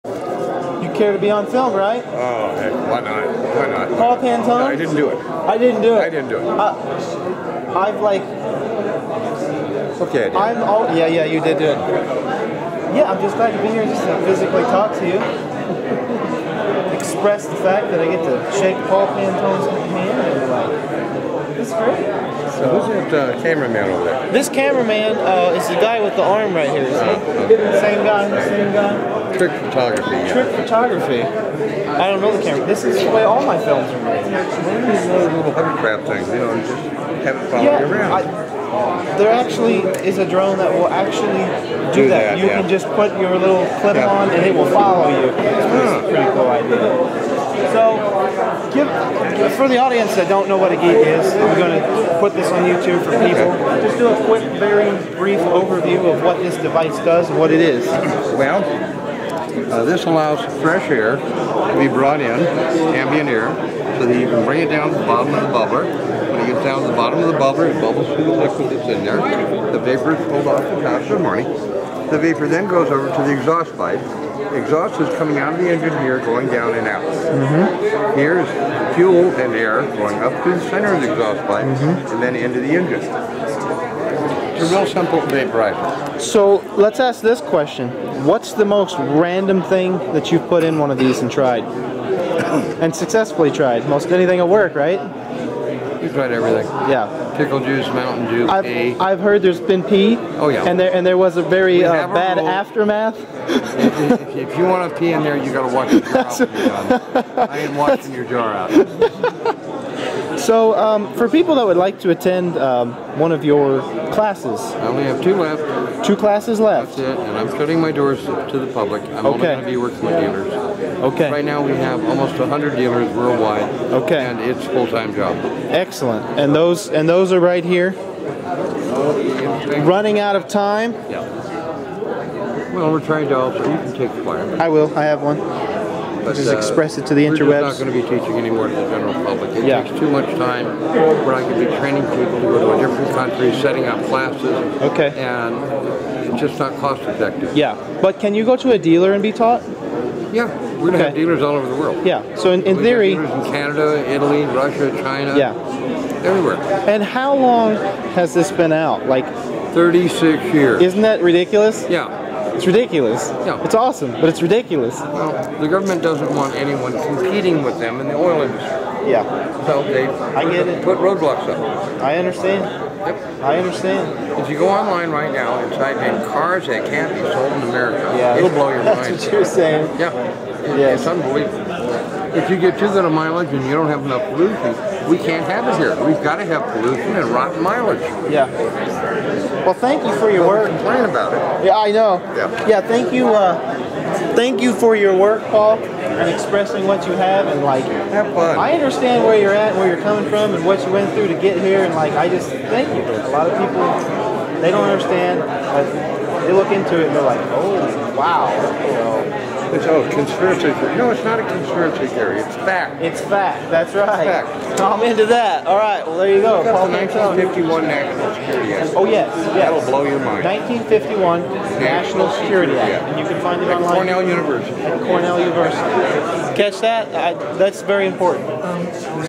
~Russ Talkes with Paul Pantone About GEET reactor... tesla tech 2014
~Russ